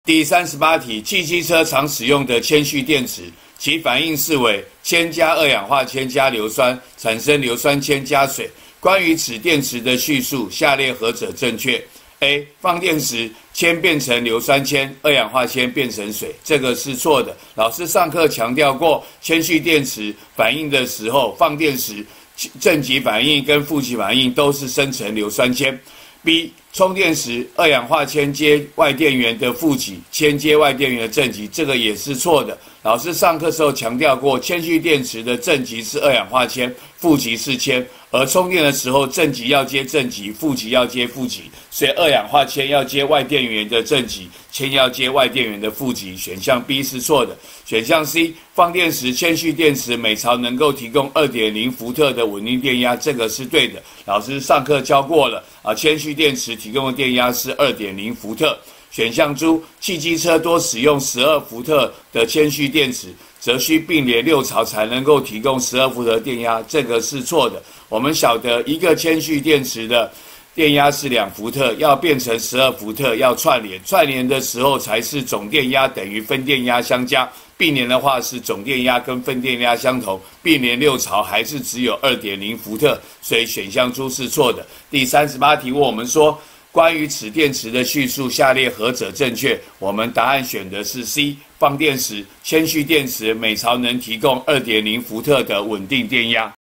108學年下9年級理化一段第38題
給鉛電池的化學反應式，問敘述何者正確。